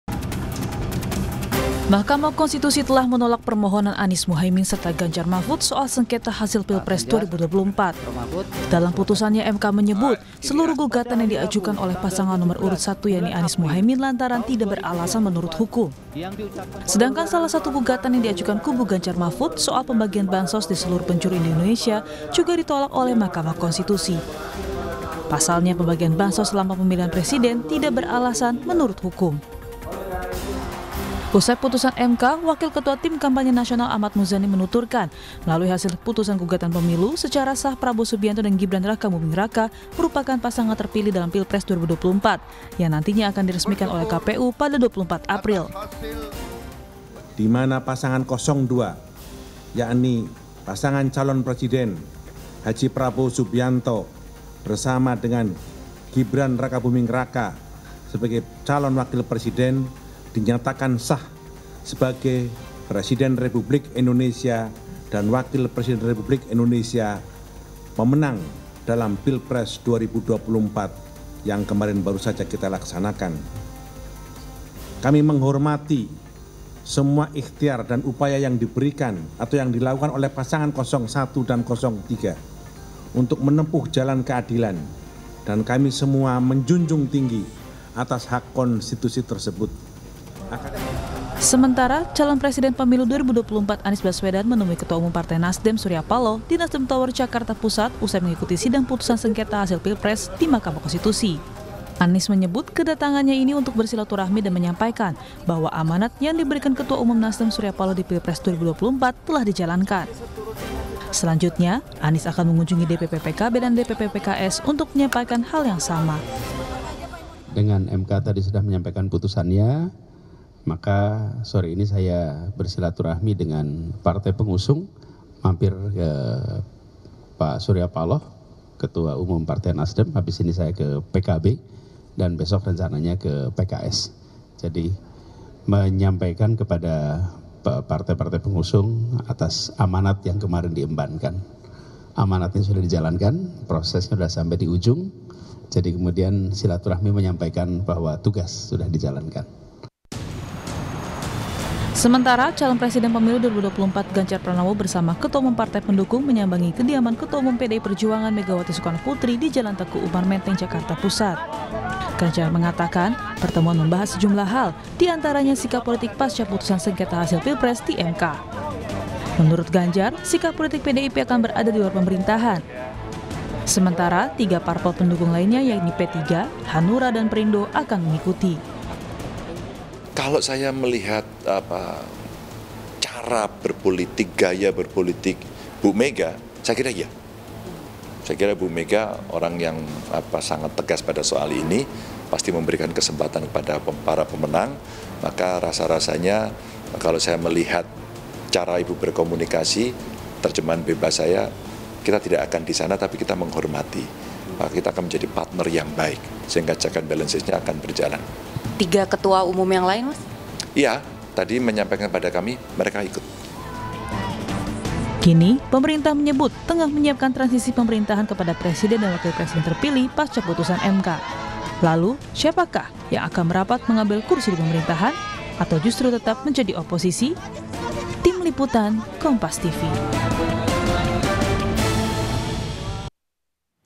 MK Tolak Gugatan Anies-Muhaimin dan Ganjar-Mahfud, Siapa yang Tetap Oposisi?
KOMPAS.TV - Mahkamah Konstitusi telah menolak permohonan Anies -Muhaimin serta Ganjar-Mahfud soal Sengketa Hasil Pilpres 2024.

Dalam putusannya, MK menyebut seluruh gugatan yang diajukan oleh pasangan Nomor Urut 1 yakni Anies-Muhaimin lantaran tidak beralasan menurut hukum.

Sedangkan salah satu gugatan yang diajukan kubu Ganjar-Mahfud, soal pembagian Bansos di seluruh penjuru di Indonesia juga ditolak oleh Mahkamah Konstitusi. Pasalnya, pembagian bansos selama pemilihan presiden tidak beralasan menurut hukum.

Kini pemerintah menyebut tengah menyiapkan transisi pemerintahan kepada presiden dan wakil presiden terpilih pasca putusan MK.

Lalu siapakah yang akan merapat mengambil kursi di pemerintahan? Atau justru tetap menjadi oposisi?

Baca Juga Gugatan Pilpres Ditolak MK, KPU Tetapkan Prabowo-Gibran Jadi Capres-Cawapres Terpilih pada 24 April di